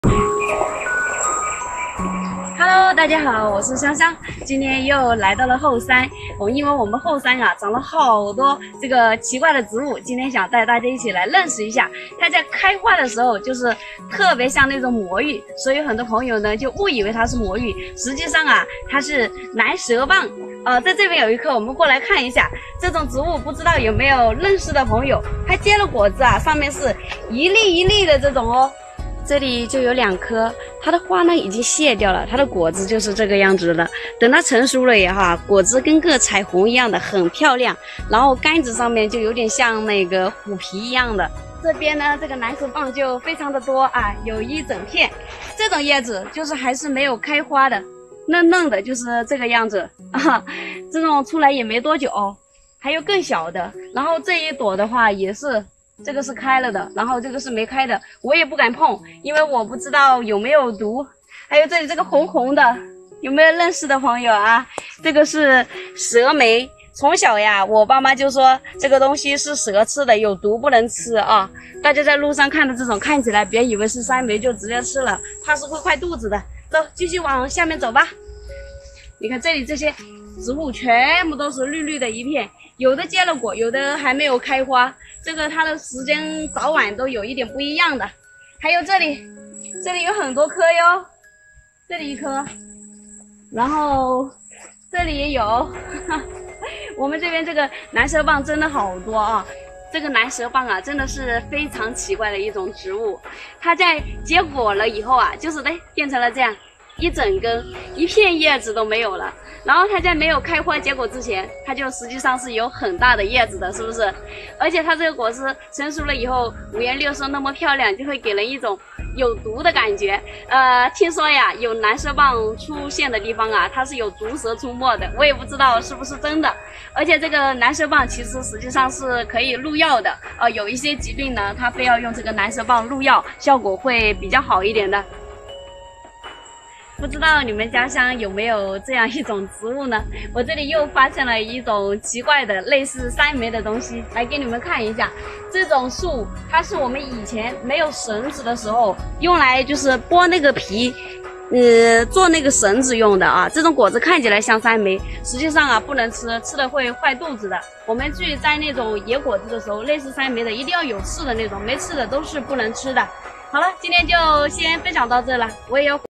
姑娘在山里发现多株南蛇棒，这种植物非常少见，这周边肯定有毒蛇
姑娘在山里发现多株南蛇棒，这种植物非常少见，这周边肯定有毒蛇,本视频由香香的日记原创提供,342次播放,好看视频是由百度团队打造的集内涵和颜值于一身的专业短视频聚合平台